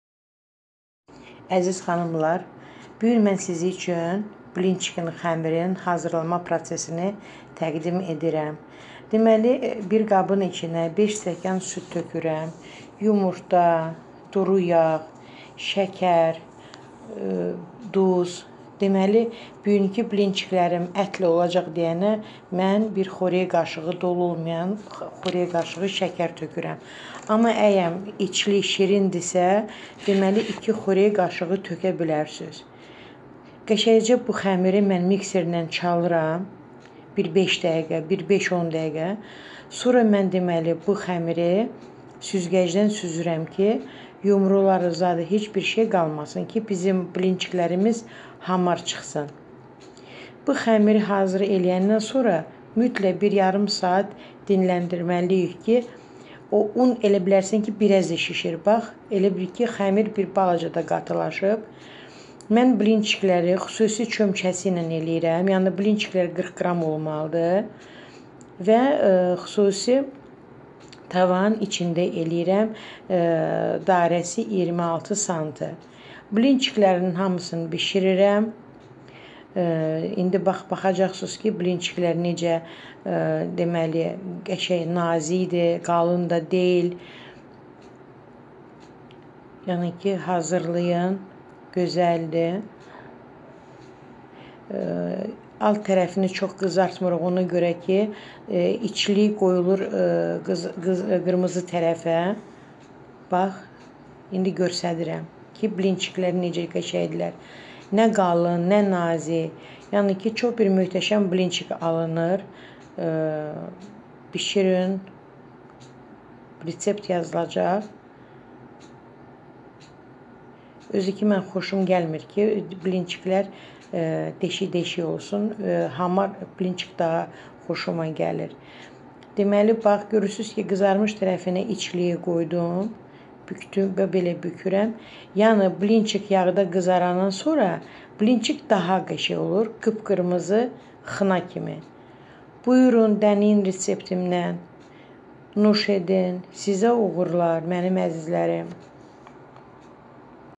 BLİNÇİK ƏTLƏ БЛИНЧИКИ С МЯСОМ. ✍🏻✍🏻✍🏻✍🏻✍🏻✍🏻✍🏻✍🏻✍🏻✍🏻✍🏻✍🏻✍🏻✍🏻✍🏻✍🏻.
RESEPT🥞 RESEPT 🥞 RESEPT 🥞 RESEPT.
 Bu porsiyadan-33 ədəd blinçik çıxır
Tavanın dairəsi -26sm.
♻️XƏMİR♻️
🥞süd-5stəkan.
🥞yumurta-3 ədəd.
🥞duru yağ-2 x.qaşığı
🥞şəkər-1 x.qaşığı
🥞duz-1 ç.qaşığı dolu olmayan.
🥞 un-3-4stəkan.
♻️ICLIK♻️
🥞tikə mal əti-1kq.
🥞soğan - 2 ədəd iri olsun.
🥞duz,istiot.
HAZIRLANMA QAYDASI:
1️⃣-əti doğrayıb,yuyun ve bişirin,bir az yağ-
da qızardın.
2️⃣-soğanı xırda doğrayıb yağda qızardıram
soyuduram və sonra ətlə birlikdə ət maşınından keçirdirəm, duz və istiot vurub, qarışdırıram,içliyimiz hazır.
3️⃣-bir qabda xəmir üçün olan ərzaqları mikserlə qarışdırıb çalıram, süzgəcdən süzürəm və blinçikləri bişirirəm,soyuduqdan sonra blinçikləri bükürəm.
NUŞ OLSUN 😊🌹❤️

РЕЦЕПТ🥞РЕЦЕПТ🥞РЕЦЕПТ🥞РЕЦЕПТ
из зтой порции выход-33блинчика.d24см.
♻️ТЕСТО♻️
🥞молоко-5 стаканов.
🥞яйца-3шт.
🥞растит.масло-2стол.ложки.
🥞сахар-1стол.ложка.
🥞соль-1чайн.ложка.
🥞мука-3-4стакана.
♻️НАЧИНКА♻️
🥞мясо гофядина-1кг.
🥞лук-2шт.большие
🥞соль,перец
ПРИГОТОВЛЕНИЕ:
1️⃣-мясо нарезать,вымоть и сварить,в конце немного пожарить.
2️⃣-лук мелко нарезать и пожарить,осту-
дить,после чего вместе с мясом пропус-
тить через мясорубку,посолить и поперчить перемешать,начинка готова.
3️⃣-в глубокой посуде все продукты для
теста смешать,взбить миксером ,проце-
дить через сито и блины готовы к жарке.
После того как блинчики остынут,блин-
чики готовы для того чтобы их заполнить
начинкой и завернуть.
ПРИЯТНОГО АППЕТИТА 😊🌹❤️